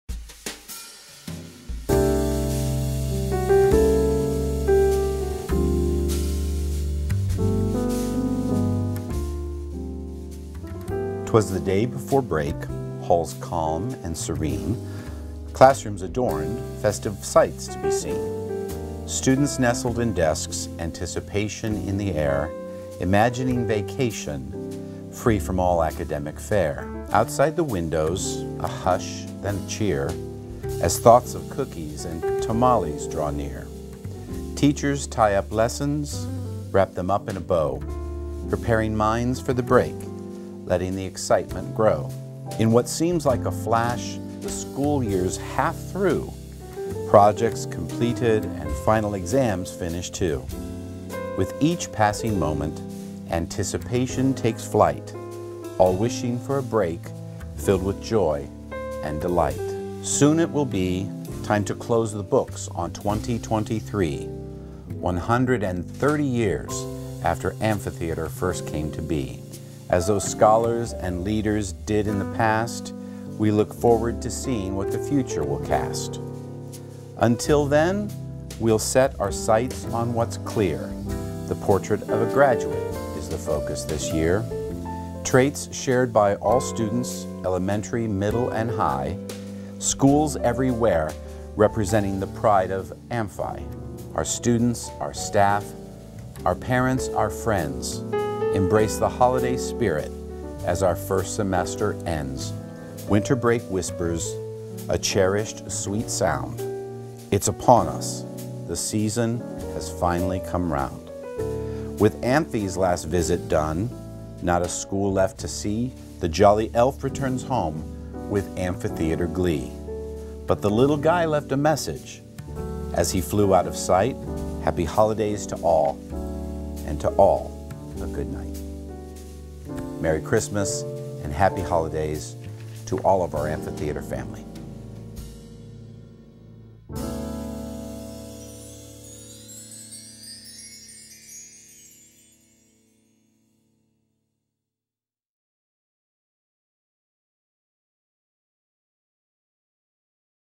'Twas the Day Before Break... - Happy Holidays Amphi 2023
Happy Holidays! Enjoy your break Amphi!

‘Twas the day before break, halls calm and serene
Classrooms adorned, festive sights to be seen
Students nestled in desks, anticipation in the air,
Imagining the break, free from books and academic flair

Outside the windows, a hush and a cheer,
As thoughts of holidays and fun draw near
Teachers with lessons, wrapped up in a bow,
Preparing minds for the break, letting excitement grow

With a jingle and laughter, the school year halfway through,
Projects completed, and exams are finished, too!
With each passing moment, anticipation takes flight,
All wishing for a break filled with joy and delight

Five months in, with classes of fun and cheer
The portrait of a graduate is the focus of the year
Traits shared by all students, elementary, middle and high
Schools everywhere representing the Pride of Amphi

Lockers echo softly, as the bell tolls the end
Winter break whispers, a cherished time to spend
In the warmth of friendship, and family so dear,
Students embrace this time, as the holidays draw near

And with Amphie’s visit complete, not a school left to see
The jolly elf returns home, filled with holiday glee
The little guy left with a shout, as he flew out of sight,
HAPPY BREAK TO ALL, AND TO ALL A GOOD-NIGHT!